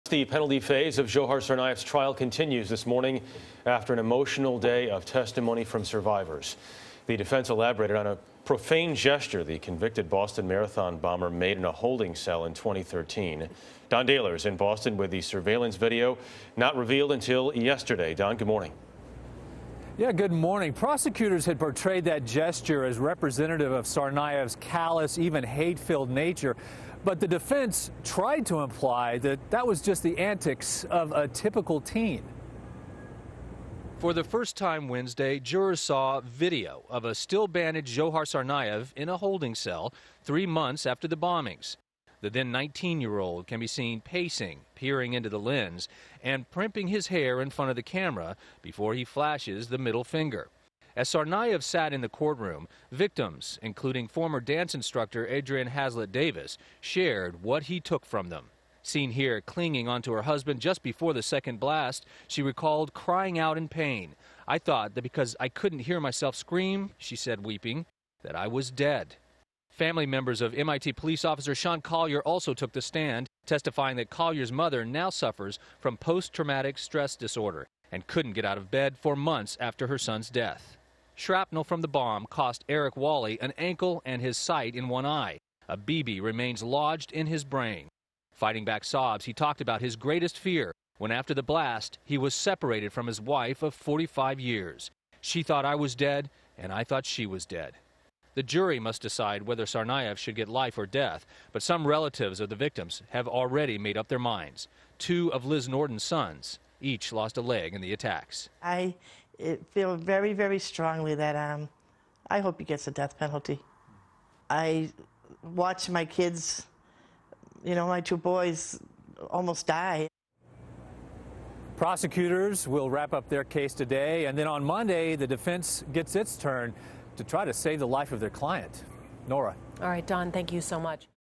Prison video of Tsarnaev emerges as Boston bomber faces sentencing
The penalty phase of Dzhokhar Tsarnaev's trial continues Thursday after an emotional day of testimony from survivors. The defense elaborated on a profane gesture the convicted Boston Marathon bomber made in a holding cell in 2013. Don Dahler reports on the surveillance video that was never seen until Wednesday.